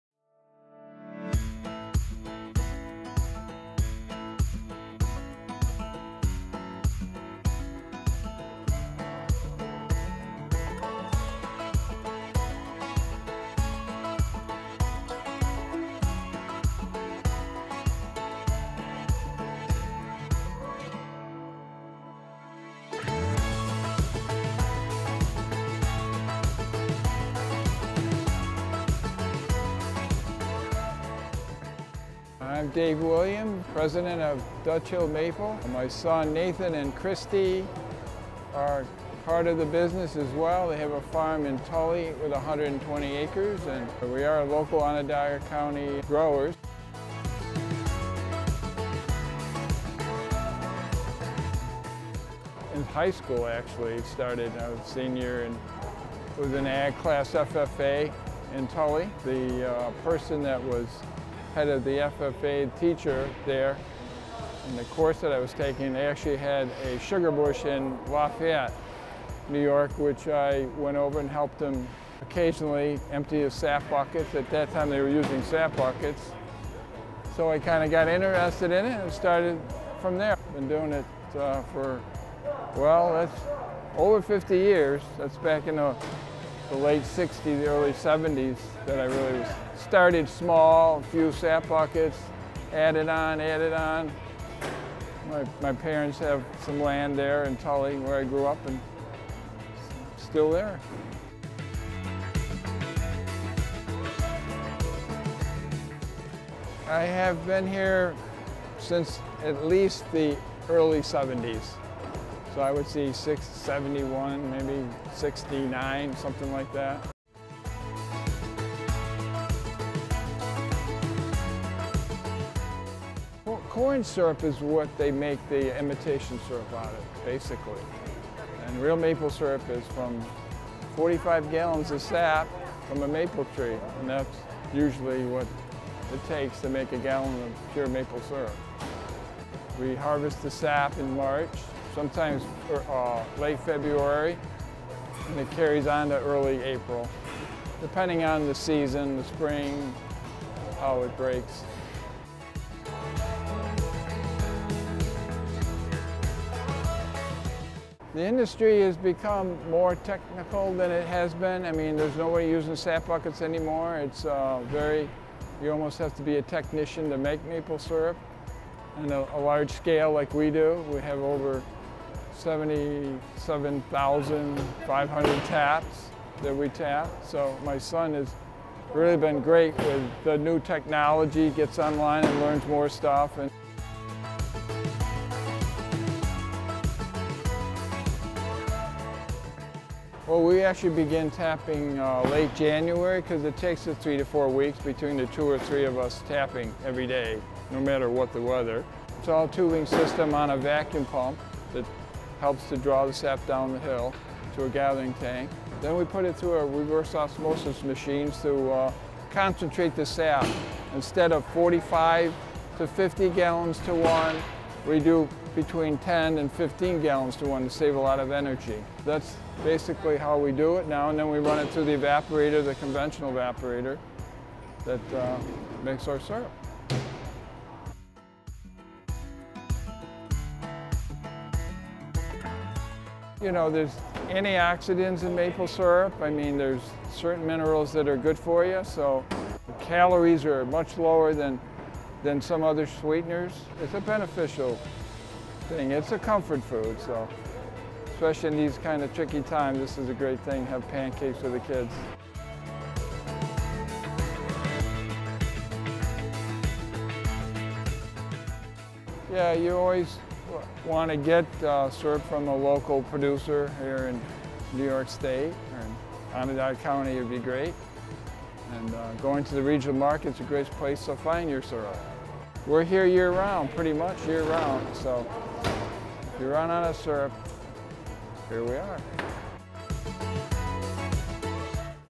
Meet the Vendor: Dave Williams
Your pancakes must be tired of sticking to imitation syrup 🥞 Dave Williams, founder of Dutch Hill Maple, will lead you to a happier and healthier breakfast. After tapping 15 maple trees in his backyard in 1968, he founded his company which would soon evolve into an entire family endeavor. But firs, he started out carrying buckets of sap through the woods to his boiling pan, where he boiled the sap down to maple syrup.

50+ years later, at their family's sugar house, Dave and his family, manage over 7,500 taps. Thanks to technological advances, they no longer primarily use buckets (although we do have a few for fun). Instead they use a tubing system, vacuum pump, and reverse osmosis.
Dave's son and business partner, Nathan, along with his wife Cristy and baby girl Kinslee, have worked to grow the family business by adding more taps, creating more maple products and providing more opportunities to visit and learn about pure maple syrup production!